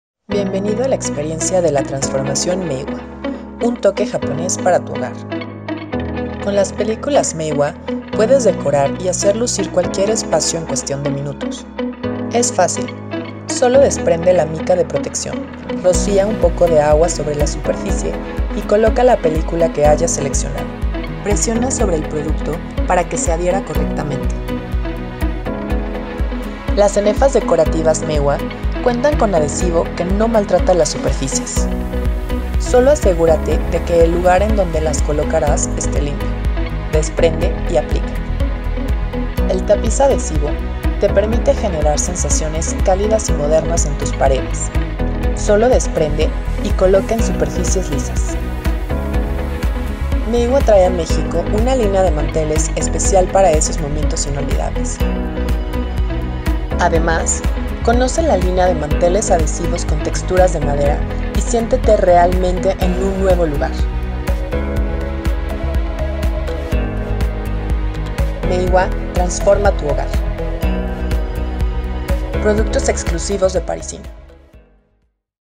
Decora con Meiwa
Conoce la línea de productos decorativos Meiwa y su fácil proceso de instalación. Lleva Japón a tu hogar.